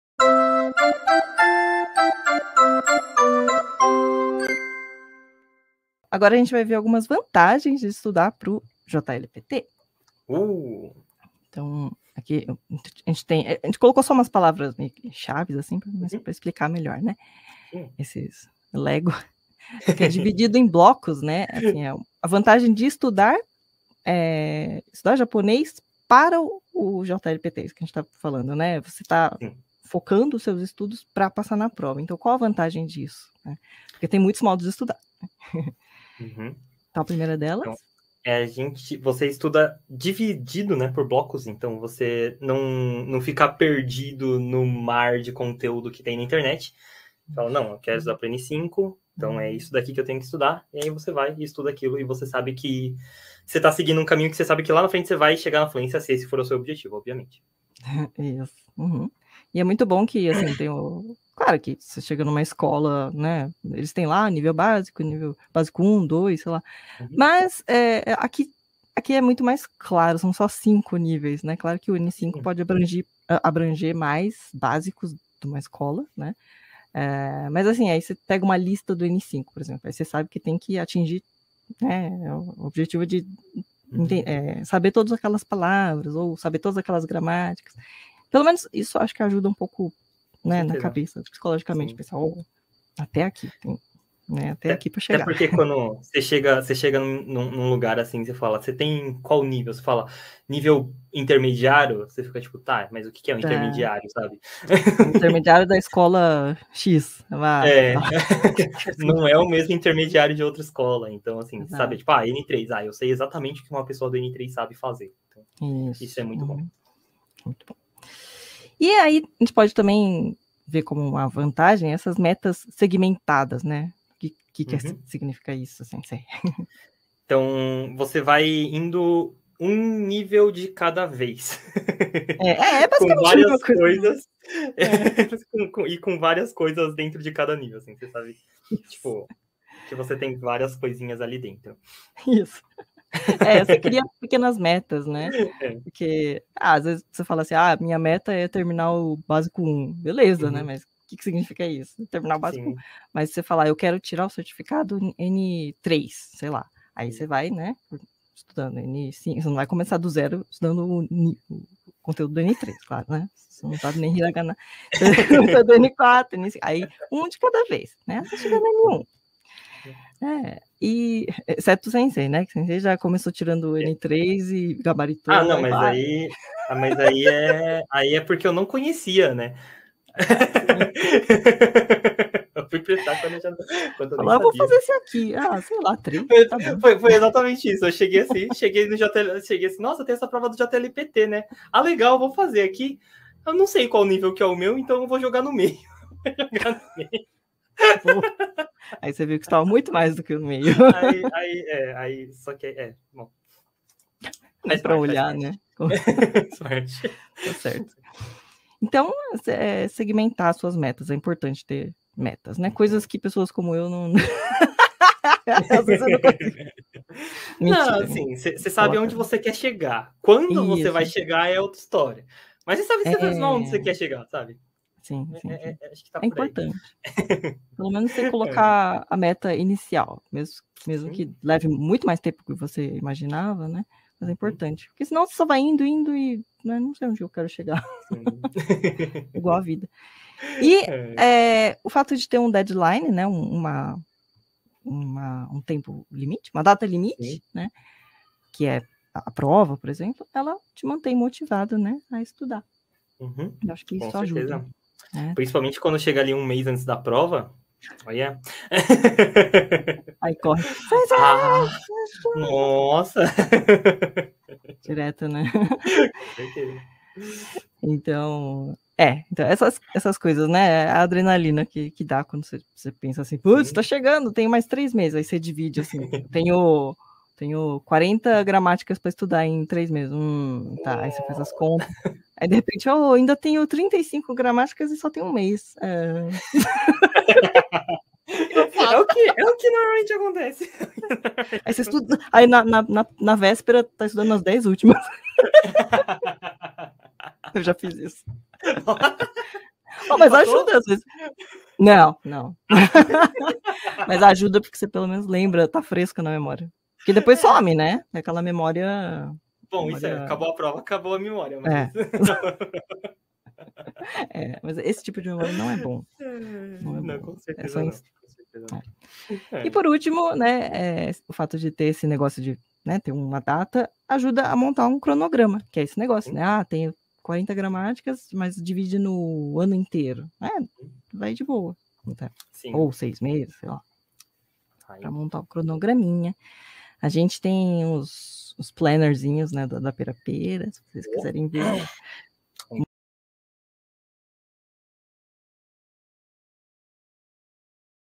Quais as vantagens de estudar para o JLPT ?
Será que vale a pena estudar para a prova de proficiência em língua japonesa (JLPT)?

Confira as respostas neste trecho da nossa live.

Bons estudos!